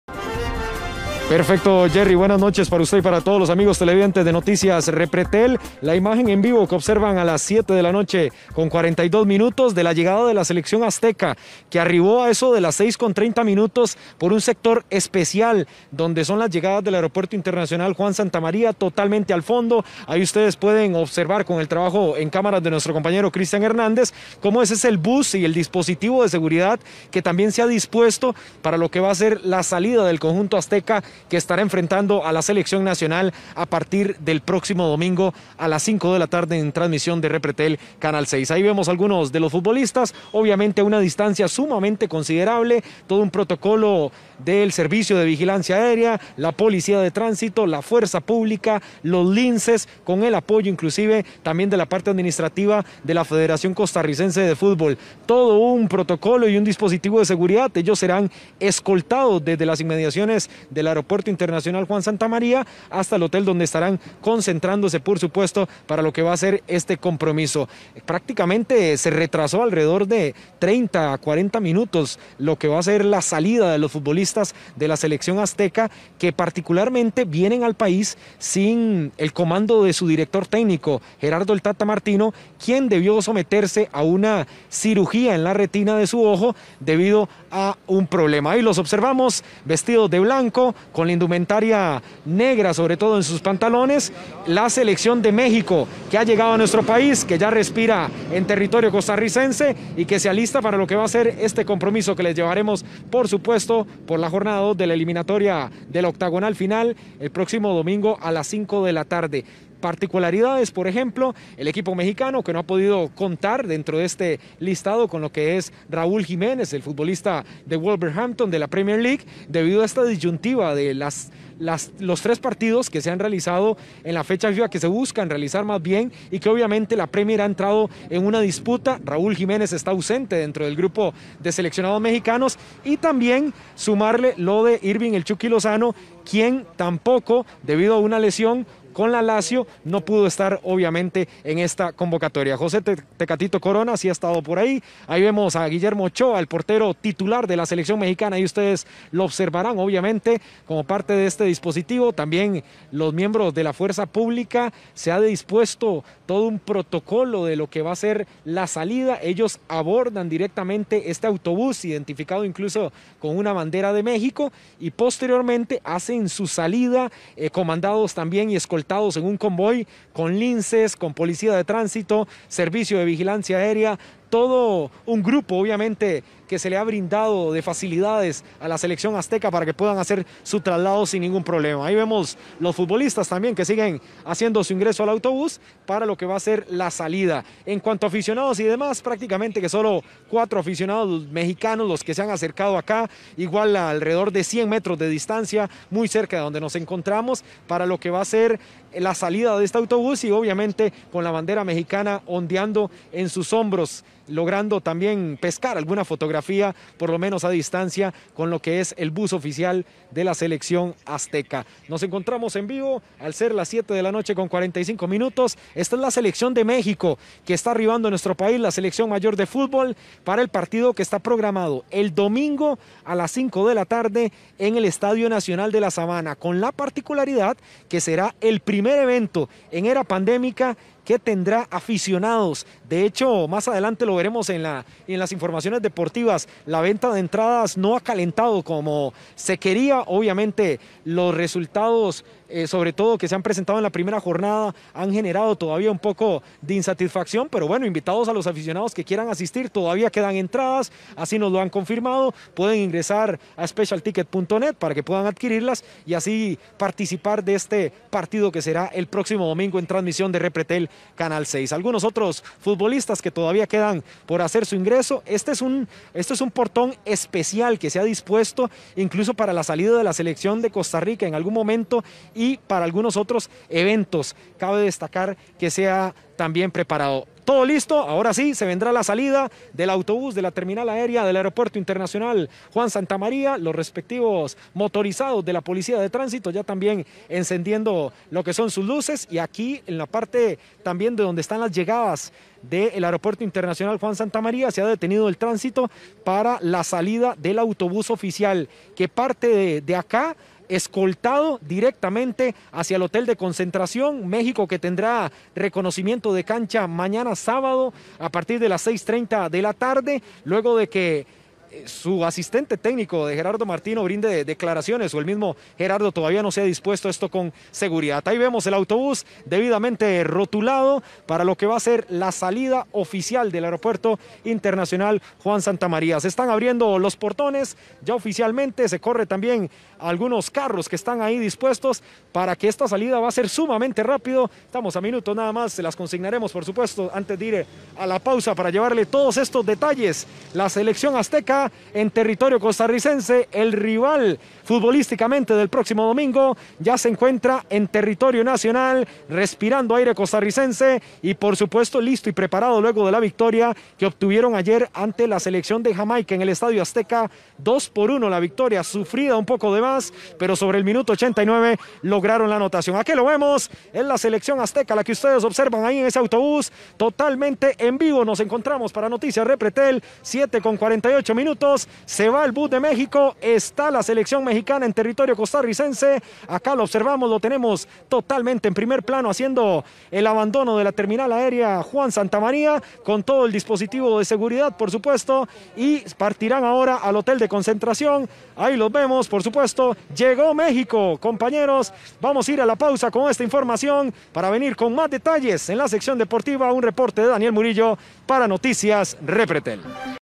Selección de México ya está en Costa Rica
La selección azteca se enfrentará a la costarricense este domingo

La selección de México arribó al Aeropuerto Juan Santamaría, en Costa Rica, para enfrentar a la selección nacional este domingo